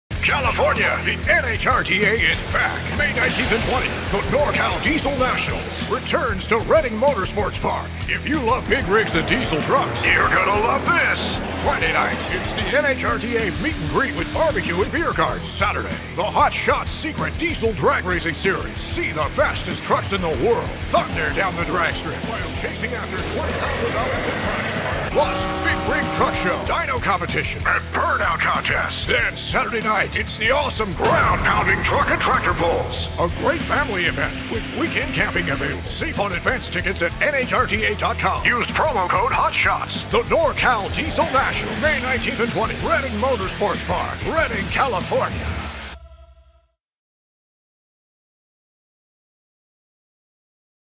The NOR CAL Diesel Nationals returns to Redding Motorsports Park
The NHRDA Presents the Nor Cal Diesel Nationals
May 19-20 at Redding Motorsports Park, Redding California

Great family event that includes Diesel drag racing, truck pulls, dyno contest, and show n shine.   Vendor row!
Plus Weekend camping available